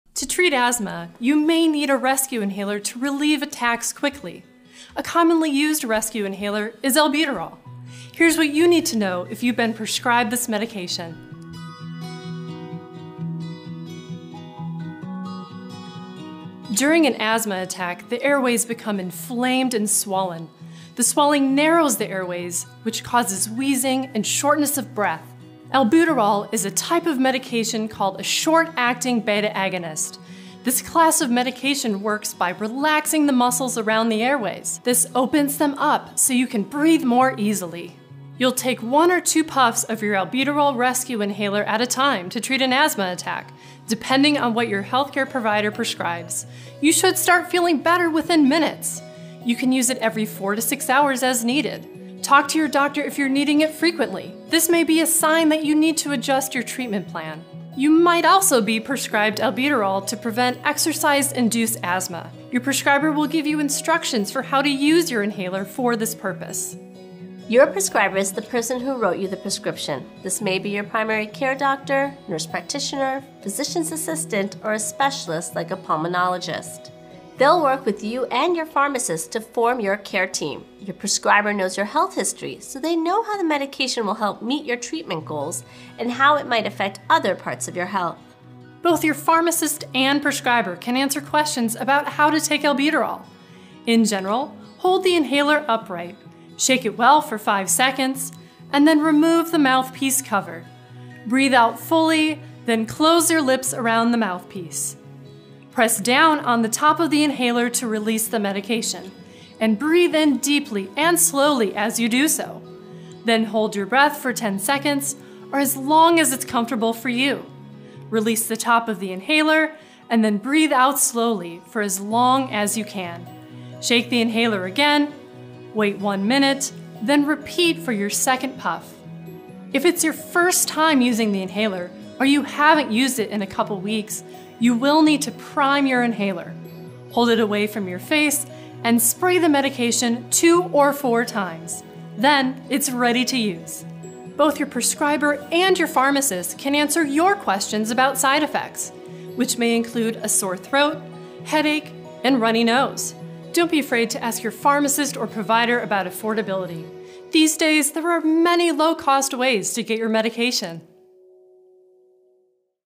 Albuterol: How It Works, How to Take It, and Side Effects | GoodRx
Albuterol (Ventolin) is a rescue inhaler that is used for people with asthma to help them breath better when they are wheezing or have shortness of breath.